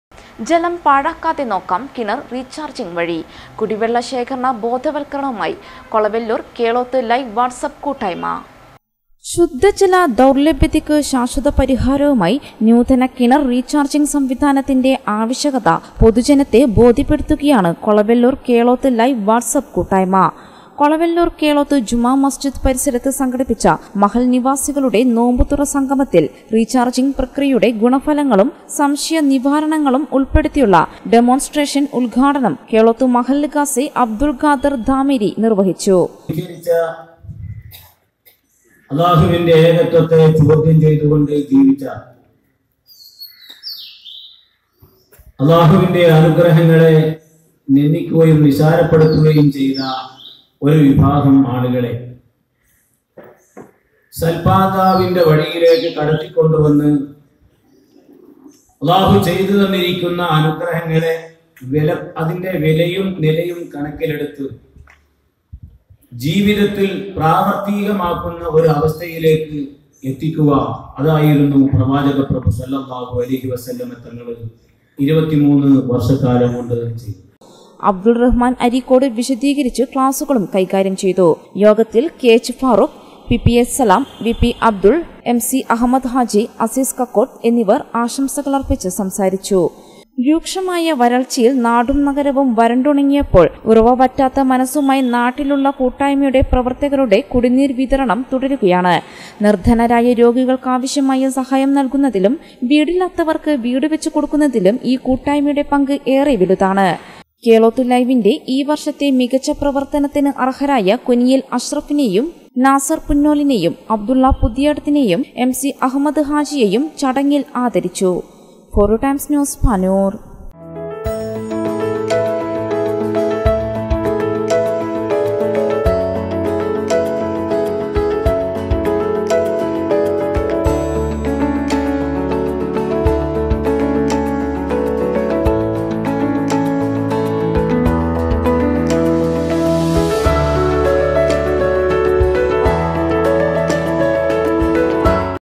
4unews
ജലം പാഴാക്കാതെ നോക്കാം കിണർ റീചാർജിങ് വഴി